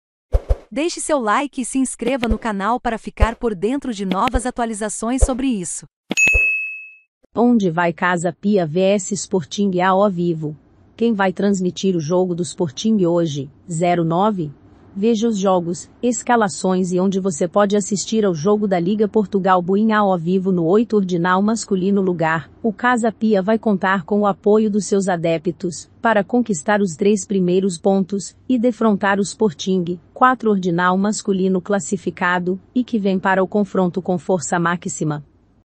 ONDE VAI CASA PIA VS SPORTING AO VIVO: QUEM VAI TRANSMITIR O JOGO DO SPORTING HOJE (09)?...
ONDE VAI CASA PIA VS SPORTING AO VIVO: Quem vai transmitir o jogo do SPORTING HOJE (09)? Veja os jogos. escalações e ONDE VOCÊ PODE ASSISTIR ao jogo da Liga Portugal Bwin AO VIVO

No 8.º lugar. o Casa Pia vai contar com o apoio dos seus adeptos para conquistar os três primeiros pontos e defrontar o Sporting. 4.º classificado e que vem para o confronto com força máxima.